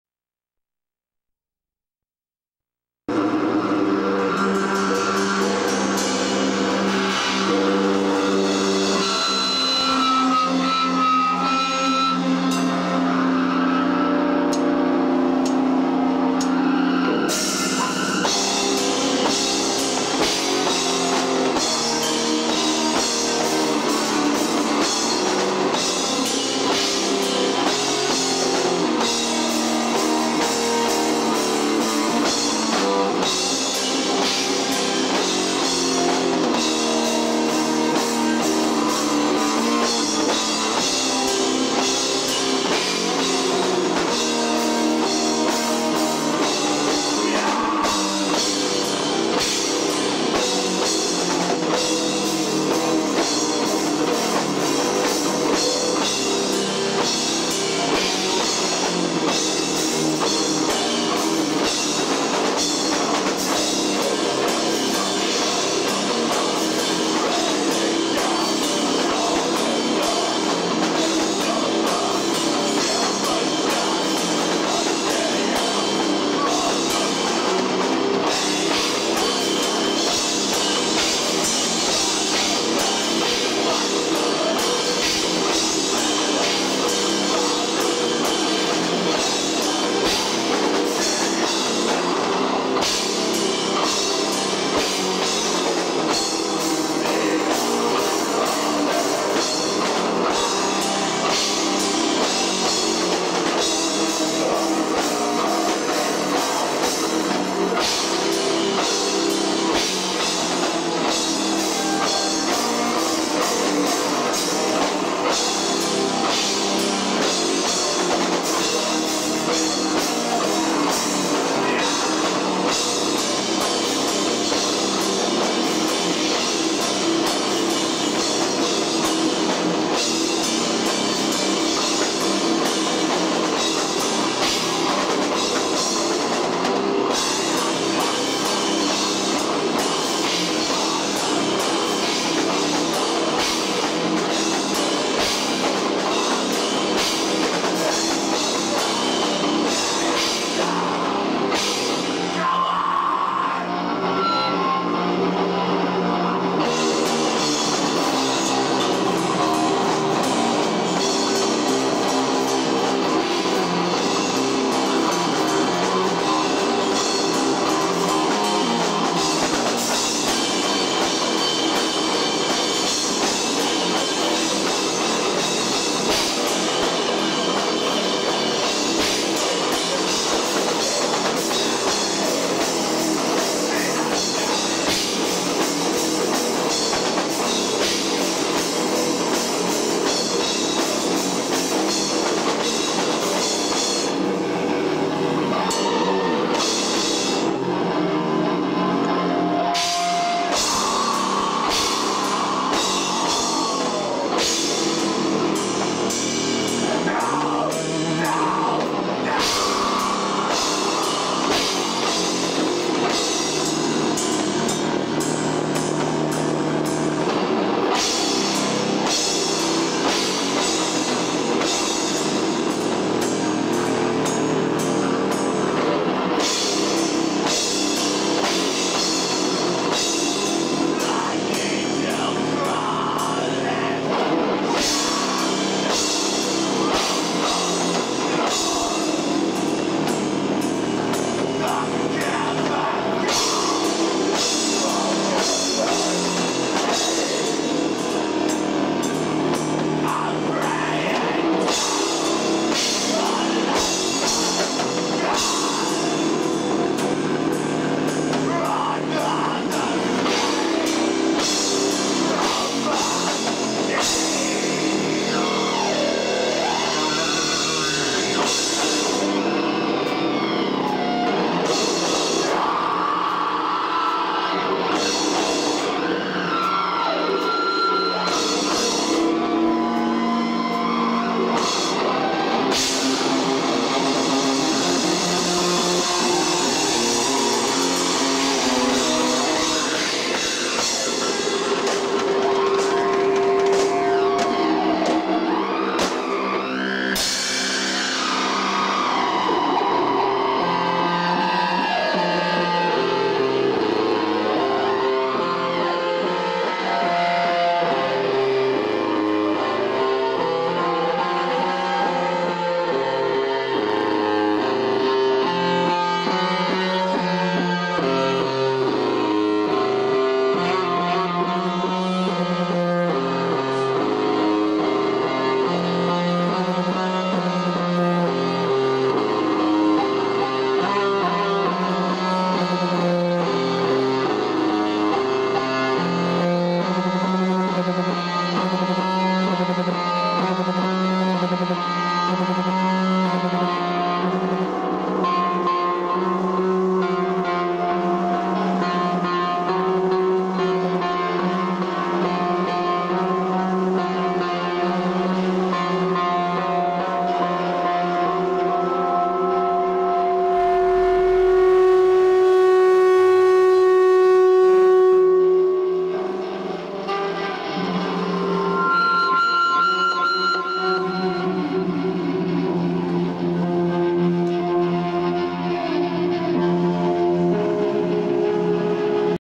Soulpreacher live part 1 at The Brewery Raleigh NC 2-28-2001 doom metal !!
Soulpreacher performing live at the Brewery in Raleigh NC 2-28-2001